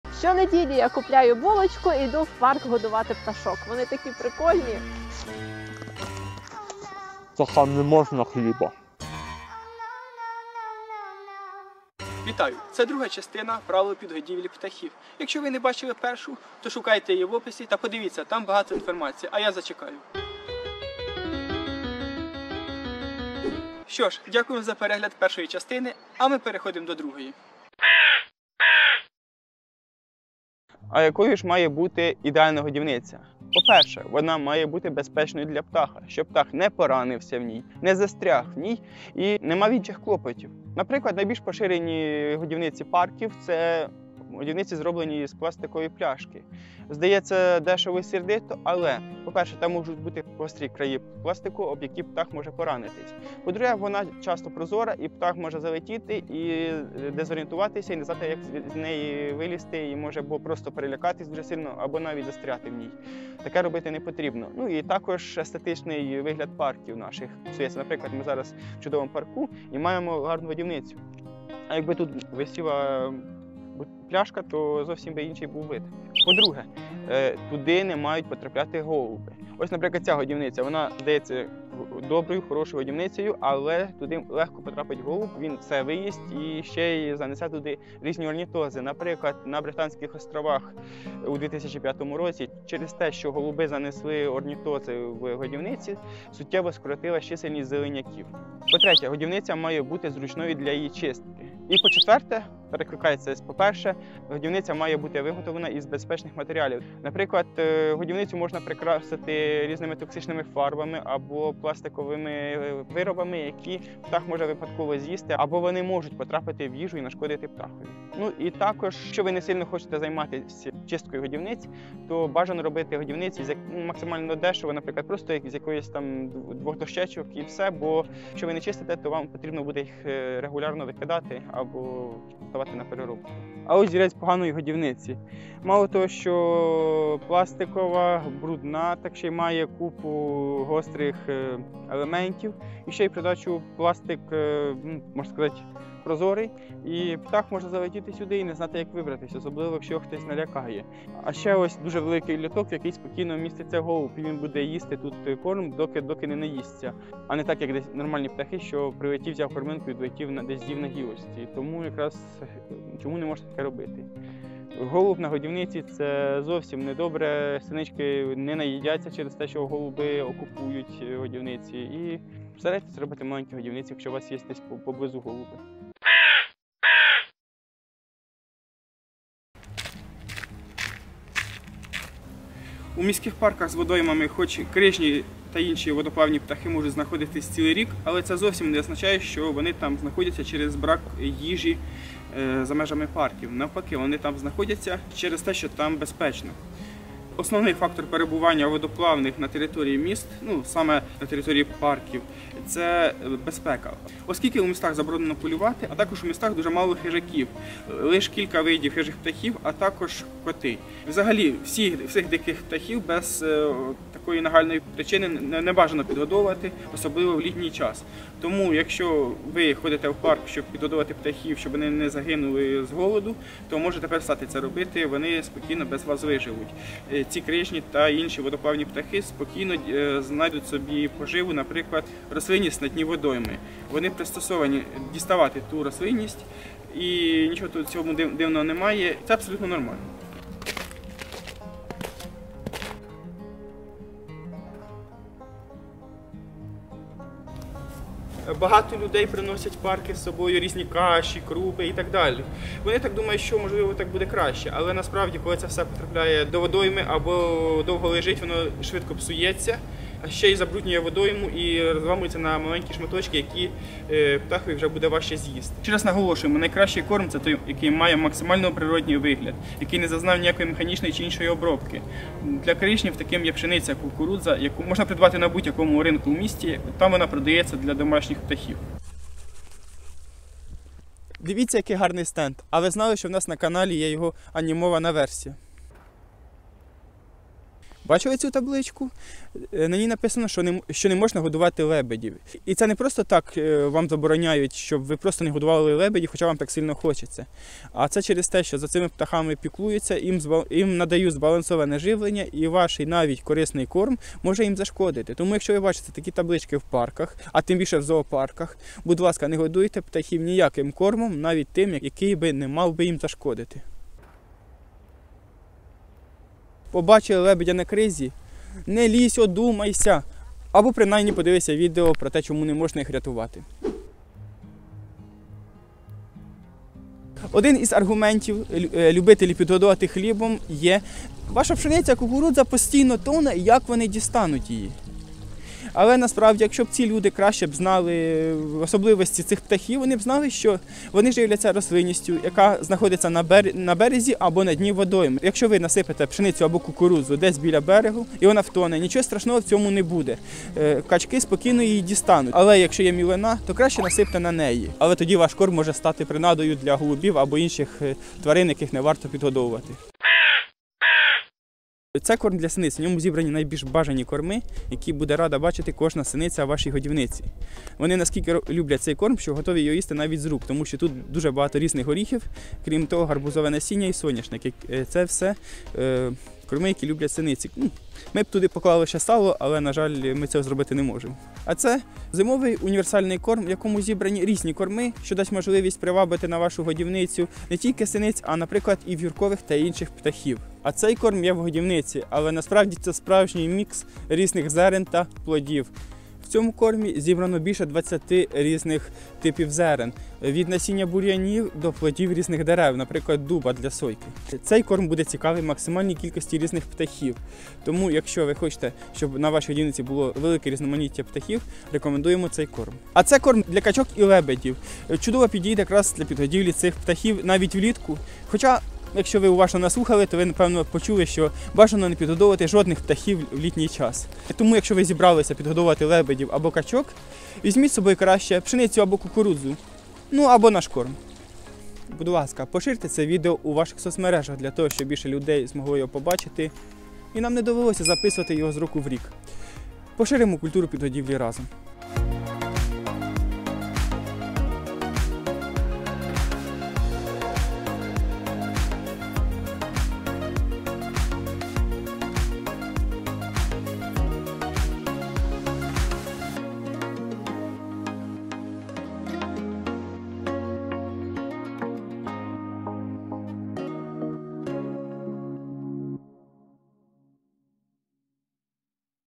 Що їдять лебеді та качки. Яка має бути ідеальна годівниця. Який корм обрати для водоплавних, синиць
Це друга частина "правил підгодівлі", ви дізнаєтеся як підгодовувати водоплавних, наприклад: лебедів та качок - щоб їм не зашкодити. Дізнаєтеся як зробити хорошу та правильну годівниць, про наш корм і не тільки. Для вашої зручності додано часові мітки:
00:00 - птахам не можна хліба
00:27 - ідеальна годівниця
02:55 - що варто знати про підгодівлю водоплавних
06:00 - про наші кормові сумішу для підгодівлі диких птахів
07:20 - подяка гостям відео та патронам проекту
✨Слідкуйте за нами:
 ✔️Станьте нашою пташкою🐦:
  😊Дякуємо за підтримку. Ви - чудові.